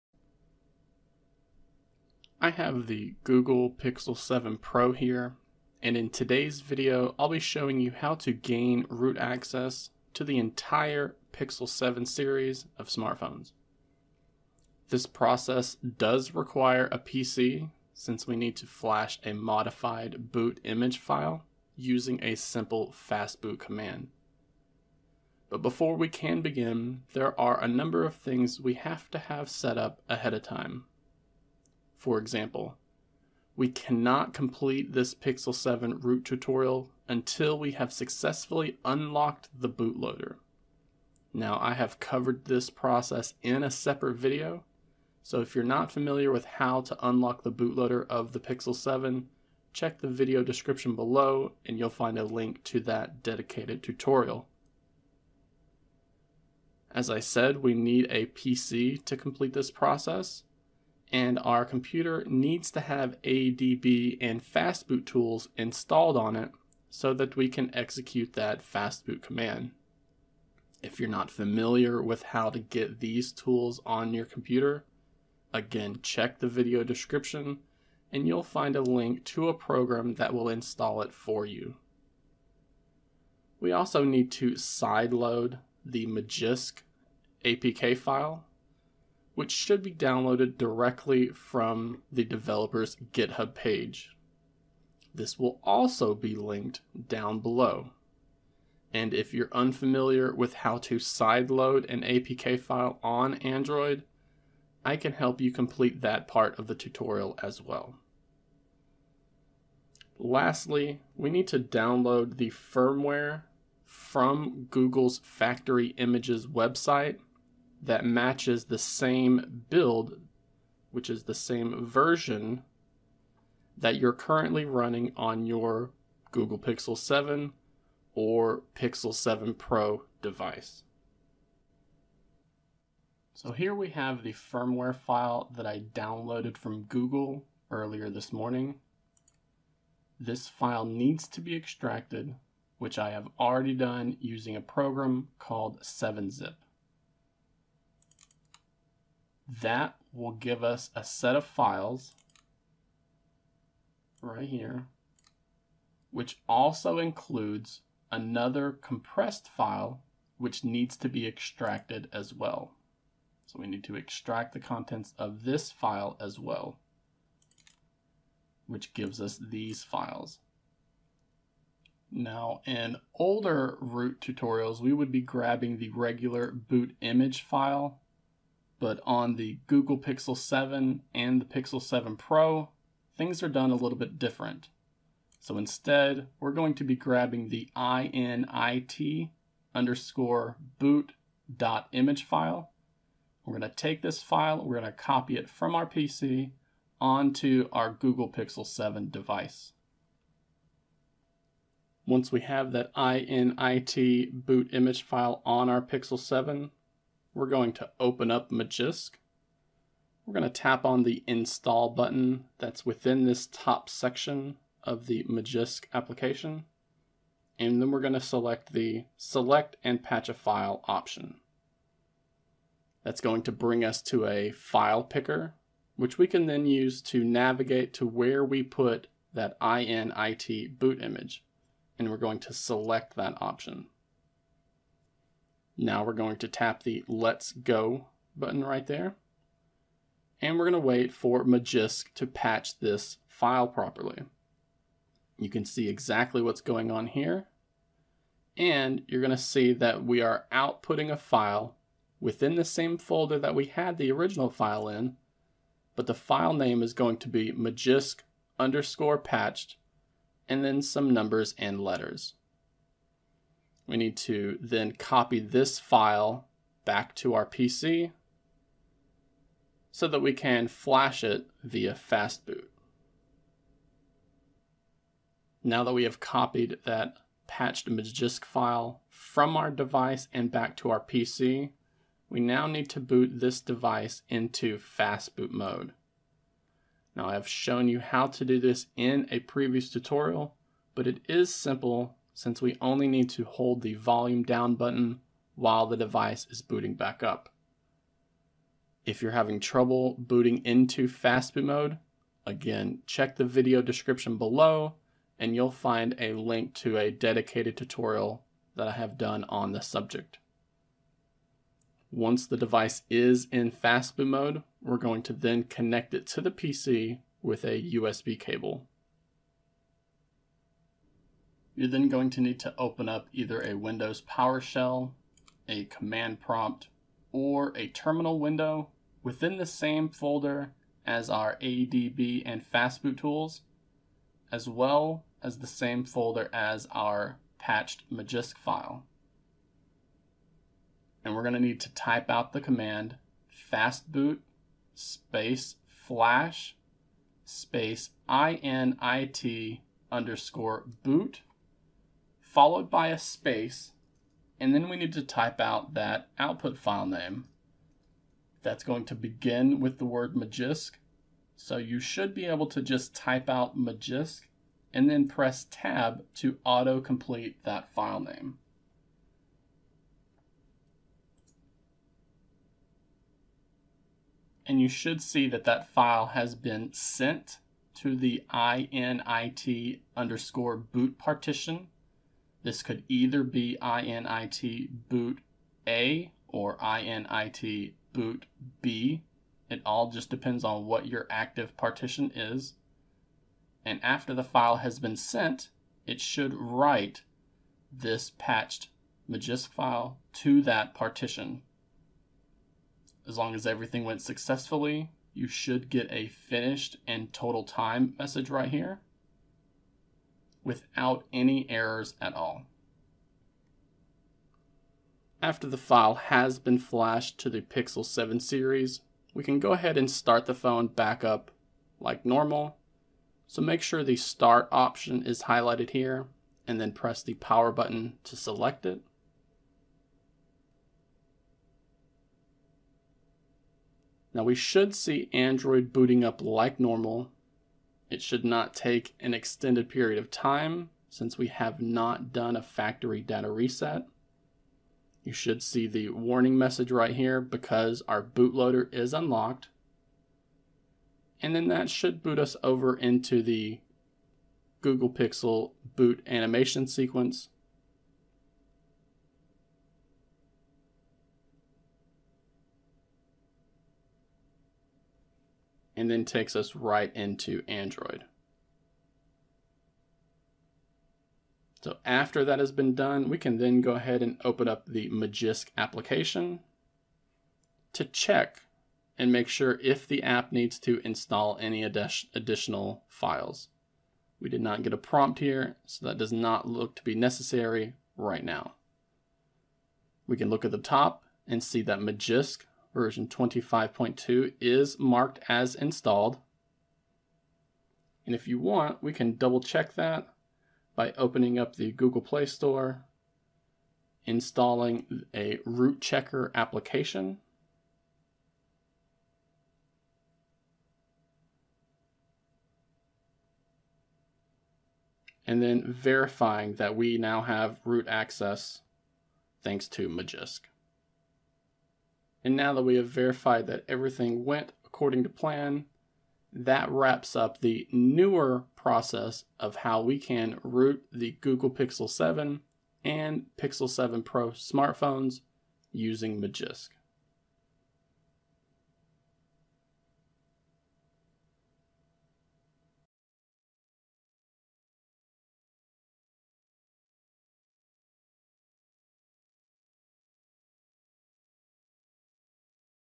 New Google Pixel 7 Root Tutorial with Magisk - Works with Pixel 7 Pro & Pixel 7a too
In this video I show you how to gain root access to the Google Pixel 7 and the Pixel 7 Pro smartphones using Magisk and a simple ADB/Fastboot command.

Video Description
I have the Google Pixel 7 Pro here and in today's video I'll be showing you how to gain root access to the entire Pixel 7 series of smartphones. This process does require a PC since we need to flash a modified init_boot.img file using a simple Fastboot command.

But before we can begin, there are a number of things we have to set up ahead of time.

For example, we cannot complete this Pixel 7 root tutorial until we have successfully unlocked the bootloader. I have covered this process in a separate video, which you'll find linked in the video description below.

Our PC needs to have ADB and Fastboot tools installed on it so that we can execute the required Fastboot command. IF you're not familiar with how to get these tools on your computer, check the video description as well and you'll find a link to a program that will install it for you.

We also need to sideload the APK for the Magisk application, which should be downloaded directly from the developer's GitHub page. This will also be linked down below as well. If you're unfamiliar with how to sideload an APK file on Android, I can help you complete that part of the tutorial as well.

And lastly, we need to download the firmware from Google's Factory Images website that matches the same build you're currently running on your Google Pixel 7 device.

So here we have the firmware file that I downloaded from Google earlier this morning. This file needs to be extracted, which I have already done using a program called 7zip.

That will give us a set of files that also includes a compressed file which needs to be extracted as well. And the init_boot.img file that we need is going to be found after we decompress that file.

Now take this init_boot.img file and copy it to your Google Pixel 7 or Pixel 7 Pro so that we can modify it with the Magisk application.

We can do that by opening up the Magisk app, tapping the Install button from within the Magisk section, and then choose the "select and patch a file" option. This will bring up a file browser which we can use to navigate to where we copied that init_boot.img file to.

After the file has been modified, we're told that Magisk has created a new file within the same folder. It should be named something like magisk_patched with some letters/numbers after it.

And we need to copy or move that magisk_patched file from our smartphone and put it on our PC within the same folder as our ADB and Fastboot tools.

Now let's boot the Pixel 7 or Pixel 7 Pro into Fastboot Mode and then connect it to the PC with a USB cable.

Open up a Command Prompt, Windows PowerShell, or a Terminal window within the same folder as our ADB & Fastboot tools.

fastboot flash init_boot filename.img

After that file has been flashed to the Pixel 7 series, we can go ahead and start the phone back up so that it boots into Android like normal. The first thing we want to do after the phone boots back up is to open up the Magisk application. It likely needs to complete the installation by performing a few more steps, which require the phone to be rebooted again.

We can even install a program like Root Checker to verify that root has been installed correctly.

That wraps up the newer process of how to root the Google Pixel 7, and Pixel 7 Pro smartphones using Magisk.

1. Intro [00:00]
2. Install Magisk [01:30]
3. Download the firmware [01:58]
4. Patch the init_boot.img file in Magisk [03:26]
5. Reboot the phone into Fastboot Mode [04:50]
6. Flash the patched file [05:40]
7. Reboot back into the Android OS [07:05]
8. Test if root has been installed properly [08:20]

Fastboot Command
fastboot flash init_boot